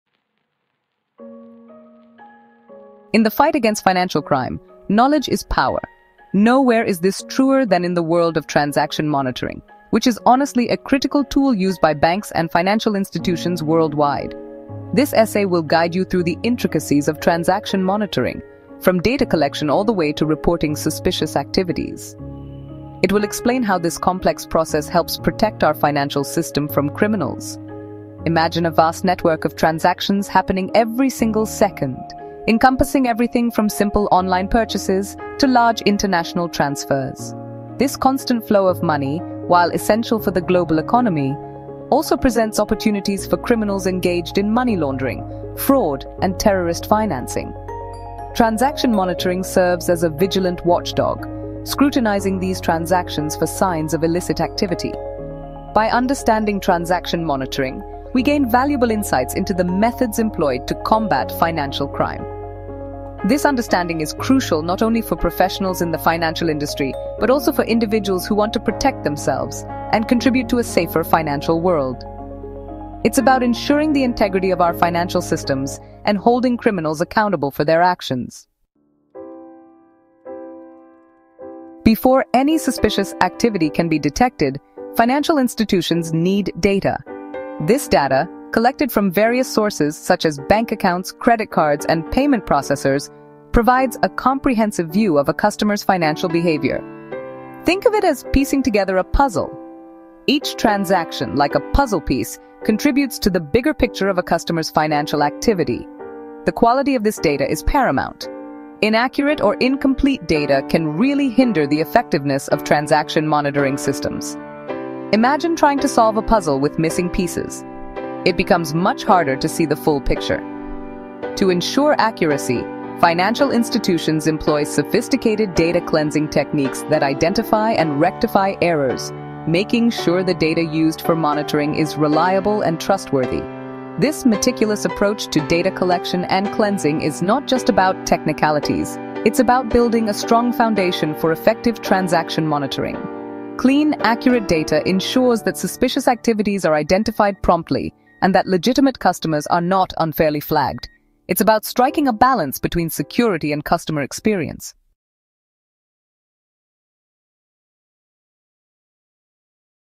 Mastering Transaction Monitoring: A Complete Guide
Dive into the intricate world of Transaction Monitoring (TM) in this comprehensive guide! Whether you’re a newcomer or a seasoned professional in Anti-Money Laundering (AML), understanding the end-to-end lifecycle of TM is essential in the fight against financial crime.

In this video, we break down the TM process, from data ingestion and scenario configuration to alert generation and case escalation. Discover how hybrid approaches—blending traditional rules with AI-driven techniques—enhance detection accuracy. We’ll also explore the importance of SAR/STR filing and the feedback loop that refines your monitoring strategies.

OUTLINE:

00:00:00 Transaction Monitoring
00:01:18 The Lifeblood of Effective Monitoring